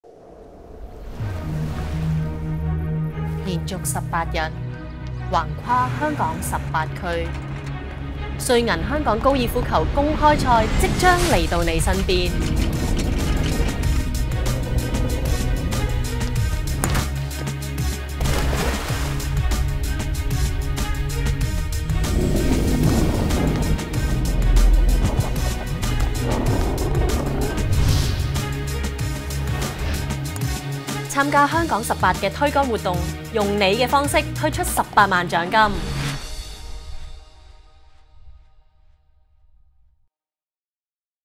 2016 UBS Hong Kong Open 瑞銀香港高爾夫球公開賽 - HK18
Putt your way to $180,000!
HK18 is bringing the 2016 UBS Hong Kong Open to a location near you. We’re on the road for 18 days, covering all of Hong Kong’s 18 districts and offering you the chance to pocket the grand prize of $180,000.

For information on HK18 dates and venues Follow Instagram: @ubshongkongopen; Twitter: @UBSHongKongOpen